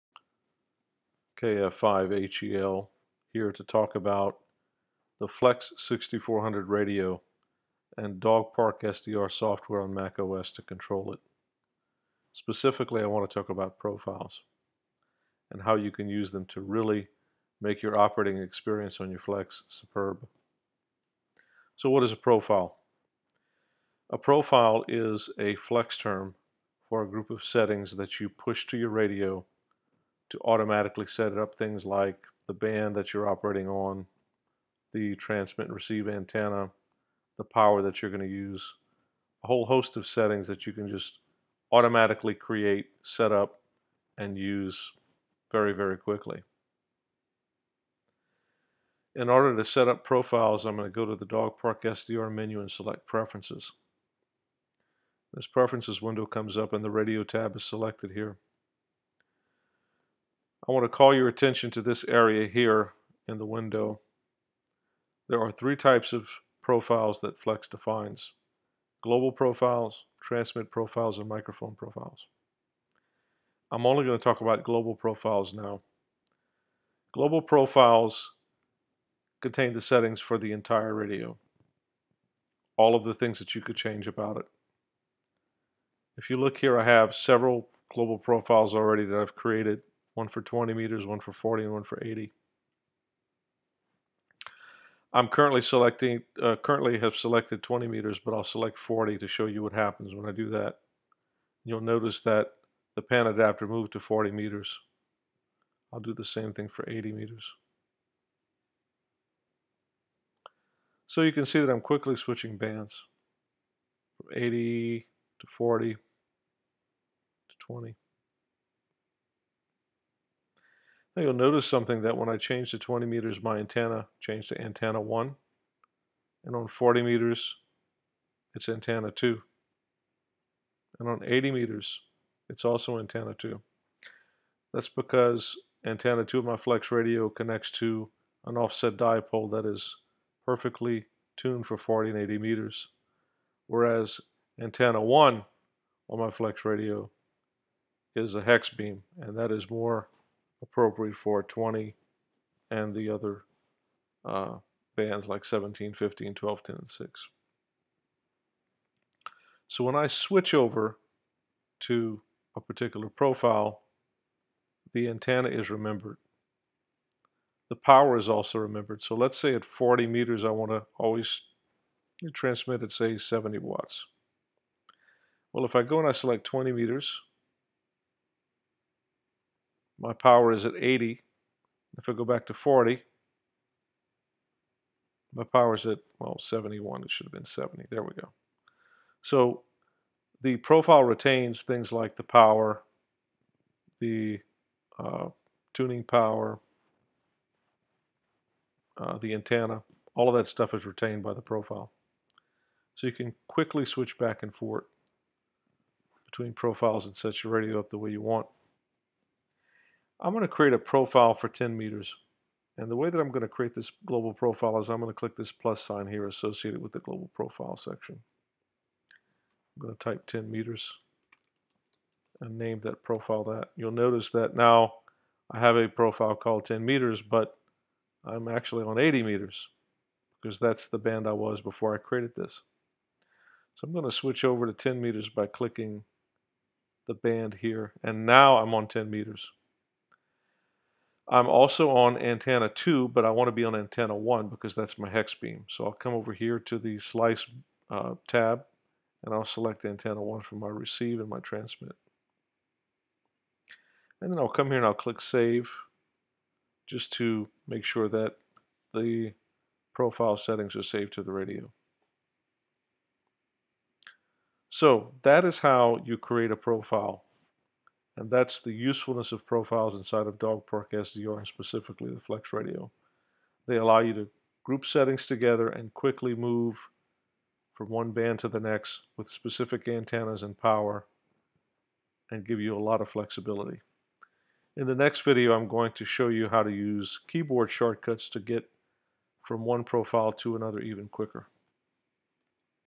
Setting up Profiles using dogparkSDR and the FLEX-6400 on the Mac
Profiles are a Flex Radio feature that provide a fast and efficient way to setup your operating environment. This video shows how to navigate dogparkSDR to manage and create profiles.

I appreciate your likes and subscribes. And pass the word around to other Mac ham radio enthusiasts!